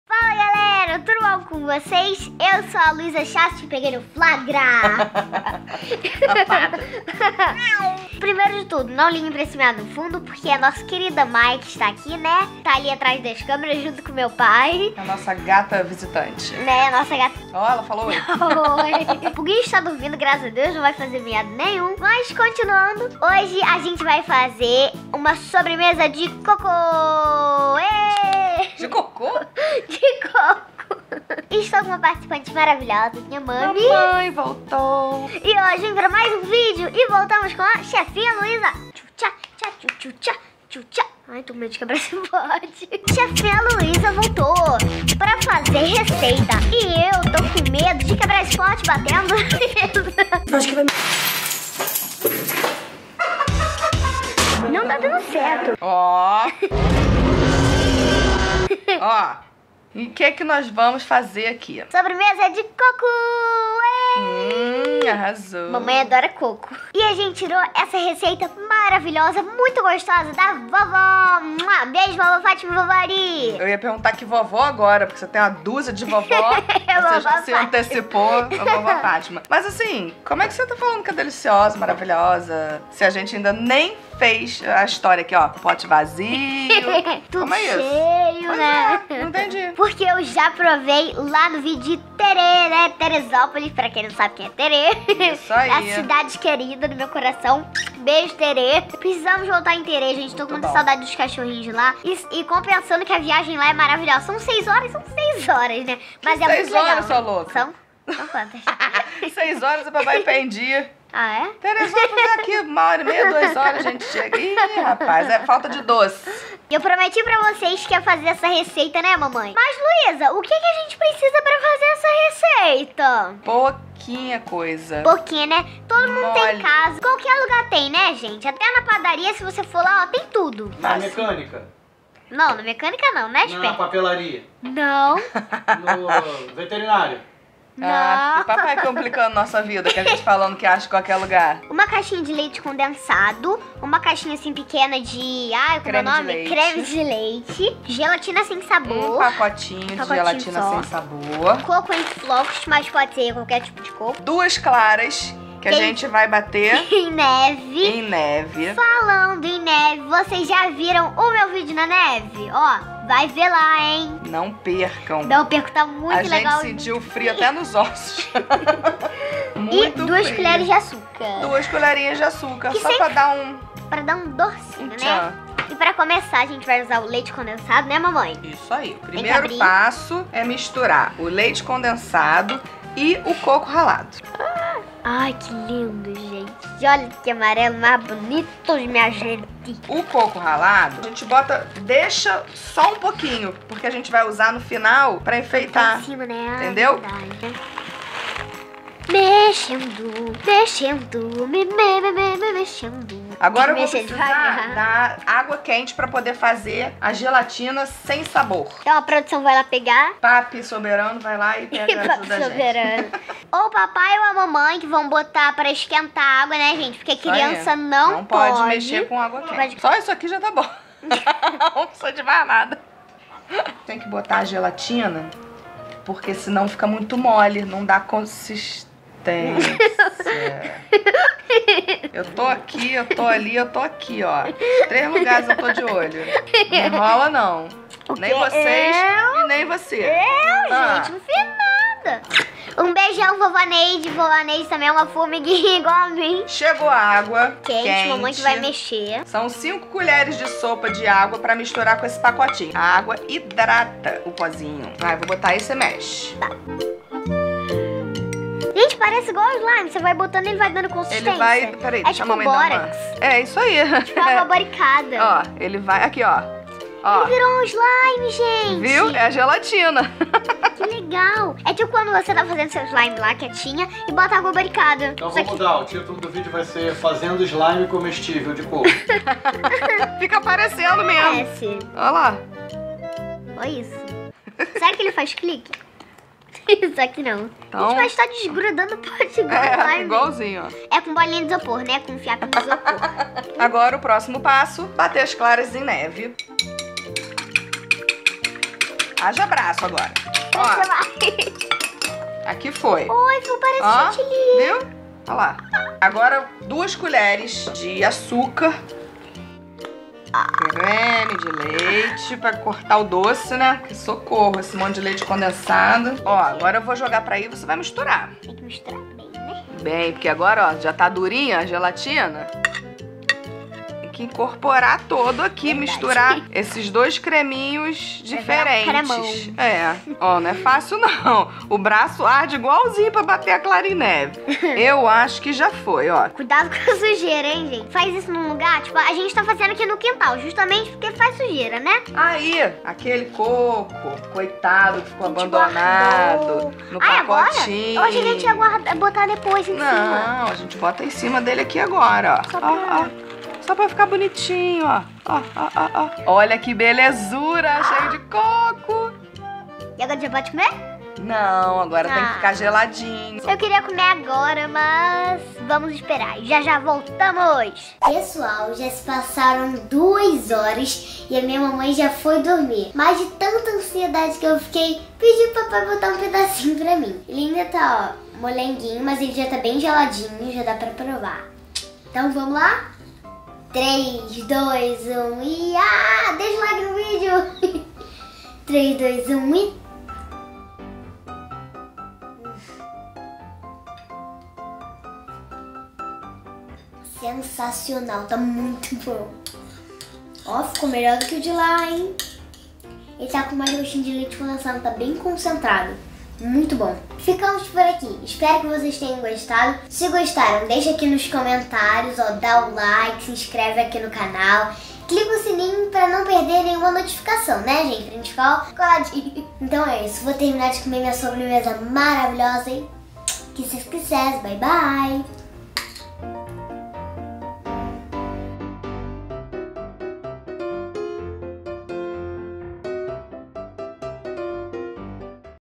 SOBREMESA DE COCO - LUISA CHASSE
Fala Galera, no vídeo de hoje vocês vão aprender comigo, Luisa Chasse, a fazer a melhor sobremesa do mundo!

Eu amooooo!

E acompanhe o meu canal porque estou sempre fazendo receitas especiais e deliciosas!